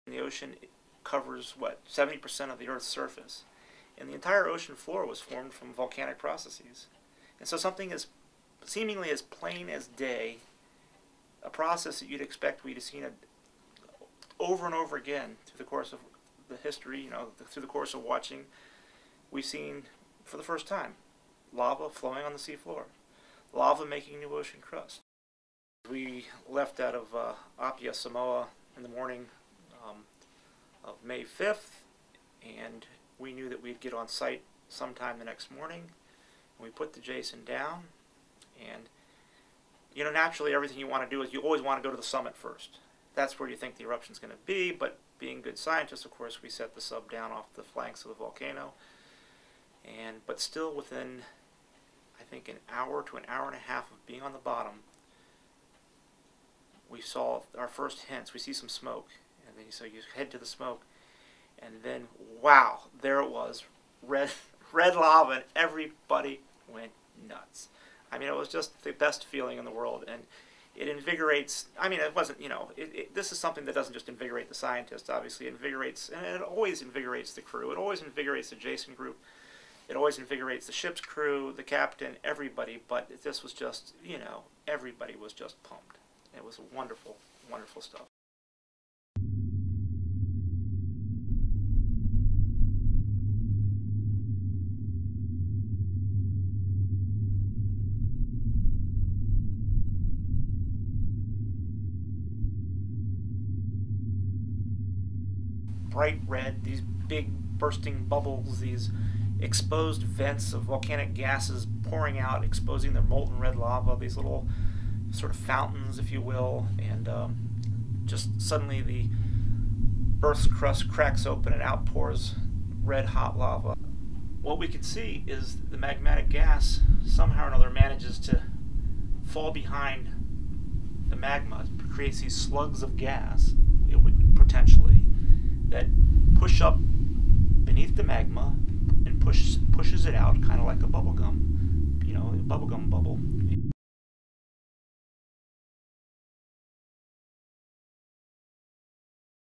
Scientists witness underwater volcanic eruption for the first time
For the first time scientists have seen molten lava flowing from a deep-ocean seafloor volcano, exploding into 35 foot long streams of red and gold and rising as bubbles as much as 3 feet across.  The expedition to West Mata volcano, near American Samoa, was led by UW Oceanographer, Joe Resing.